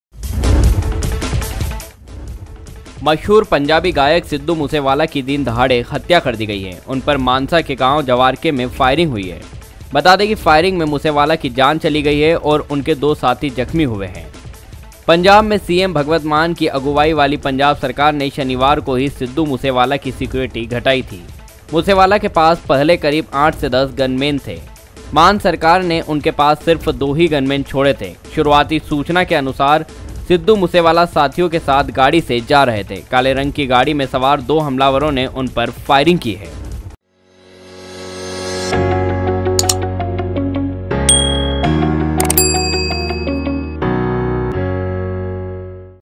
Sidhu Moosewala Firing Updates | पंजाबी गायक Sidhu Moosewala की दिनदहाड़े हत्या | Mradubhashi
शहूर पंजाबी गायक सिद्धू मूसेवाला की दिनदहाड़े हत्या कर दी गई है। उन पर मानसा के गांव जवाहरके में फायरिंग हुई। फायरिंग में मूसेवाला की जान चली गई और उनके 2 साथी जख्मी हुए हैं।